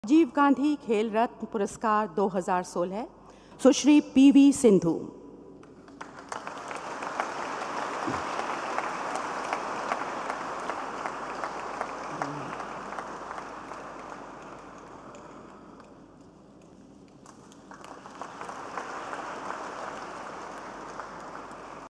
Bharat ki sherni Pv sindhu is in action. pv sindhu. # badmiton.
Pv sindu is in acttion. Pusarla was awarded the Arjun Award by the government of India. In the 2013 Indian Badminton League, she was the captain of the team Awadhe Warriors. Her team qualified for the semi-final, where they beat Mumbai Marathas, but lost in the final to Hyderabad HotShots.